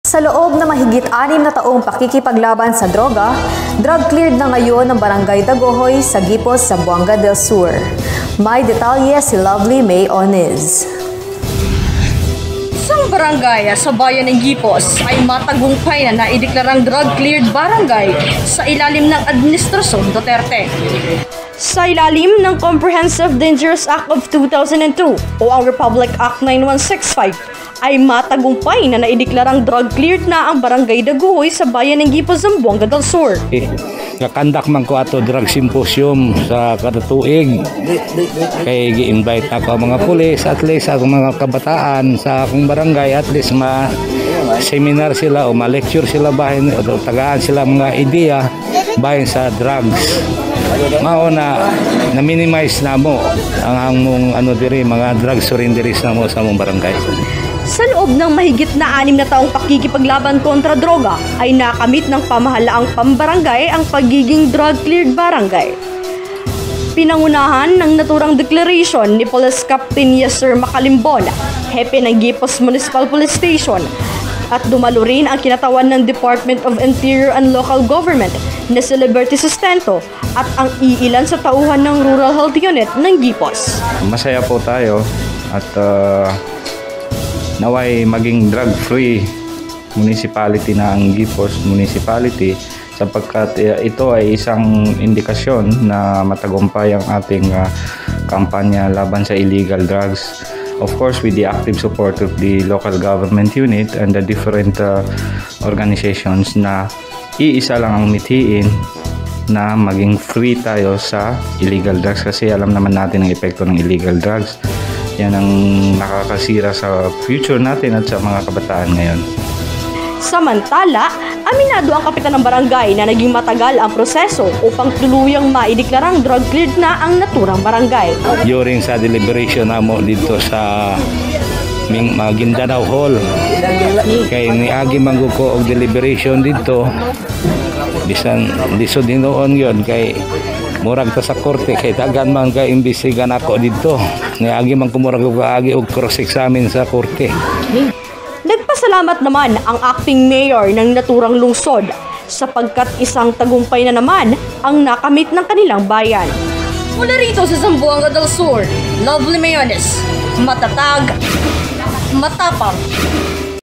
Barangay Dagohoy sa Guipos, Zamboanga del Sur, drug-cleared na
Isang barangay sa bayan ng Guipos, Zamboanga del Sur ay naideklara nang “drug cleared barangay’’.

Sa ilalim ng Comprehensive Dangerous Drugs Act of 2002, o ang Republic Act 9165, ay matagumpay na naideklarang drug-free ang nasabing barangay.